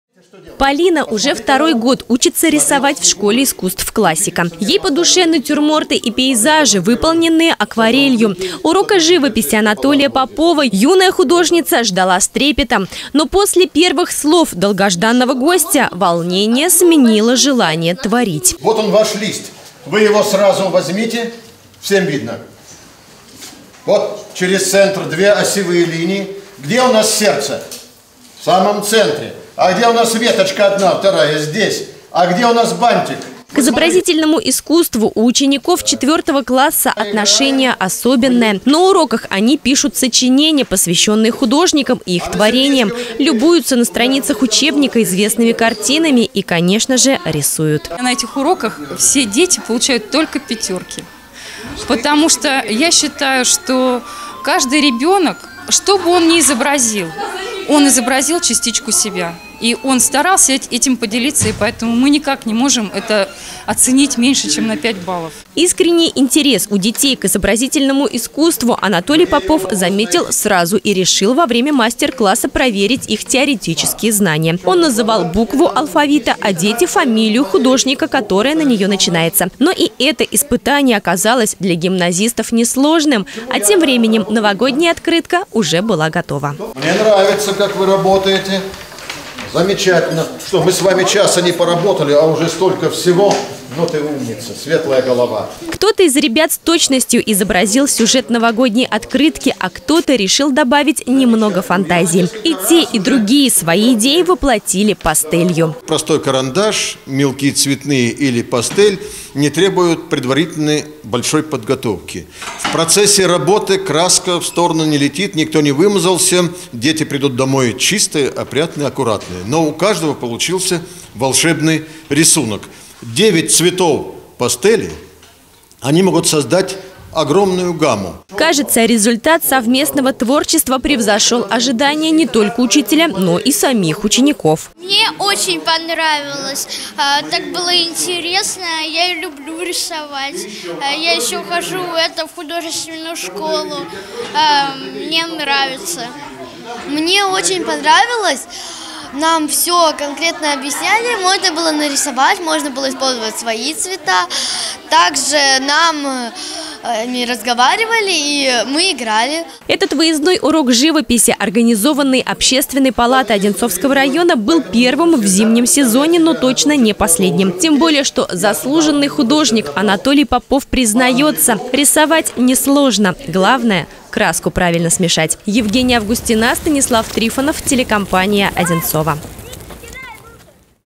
Необычный урок живописи в 11 гимназии
Готовиться к новому году ученики 11-ой гимназии начали уже в первых числах декабря. Открытки для близких ребятам помогал рисовать профессиональный художник Анатолий Попов в рамках проекта «Школа живописи. Прекрасное  своими руками». Его организовала общественная палата Одинцовского района. Как гимназисты усвоили урок, узнала наша съемочная группа.